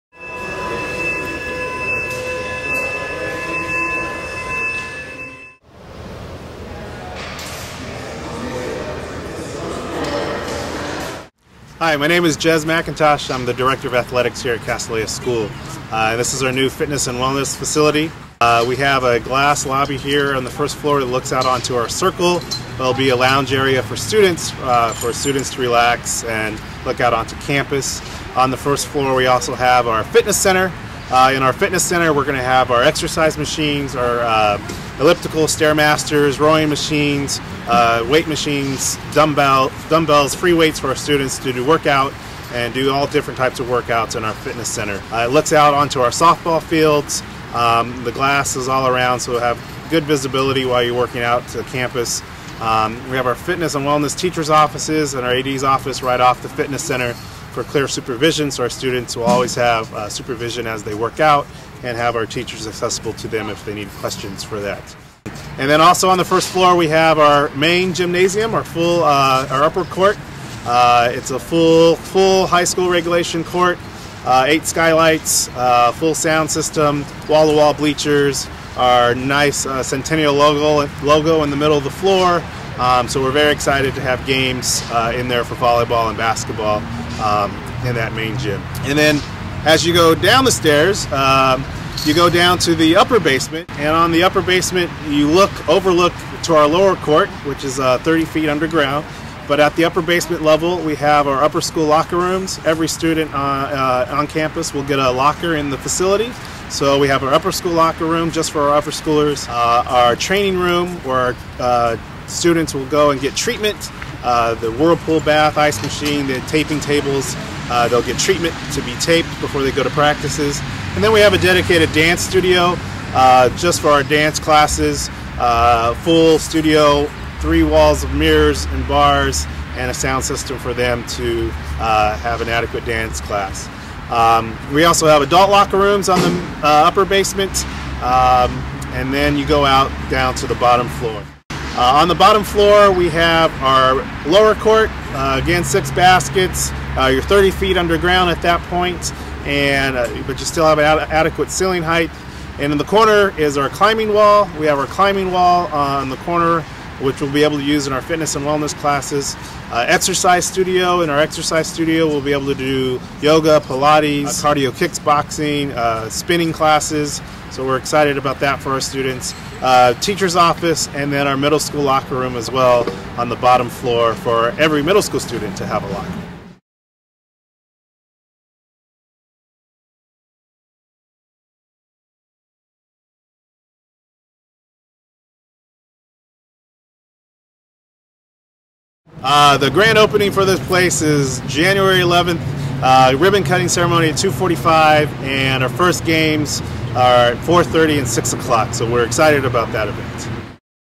Castilleja gym
Jez McIntosh, Director of Athletics for Castilleja School, offers a vocal and visual tour of the school's new $14.5 million athletics facility.